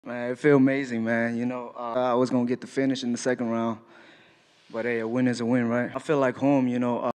Joshua Van: I’m going to get finish him in the second round 🔥
Taila Santos vs Erin Blanchfield
UFC flyweight women match

Taila Santos from Brazil, she is the former contender for the title, that loss against Shevchenko, until this loss she was 4 win streak. Santos debut in ufc in 2019 against te Italian Borella and loss the fight, but after this how I said before, she loss the opportunity to won the title, in his career she has 19 and only 2 loss. Taila Santos is a great grappler that got into trouble Shevchenko, she is top 3 ranked in ufc.

Erin Blanchfield from USA, she is the former Invicta FC fighter. Erin Blanchfield has only one defeat in his career, she is still undefeated in ufc with 5 win in a row and 8 win streak in his career in this moment, she one of the youngest fighter now, that is on the high position of the rankings she is top 3 ranked in ufc, and can possibly be the contender for after this match, a great grappler who has 60% rate finishes by submission.

For this match I see one of the highest quality match in ufc women match, between two great grappler, this prediction is not easy, because we have the contender for the title and Blanchfield which is on the rise of his career, but I want to say that this match gonna win Blanchfield, because she continued to be in rise.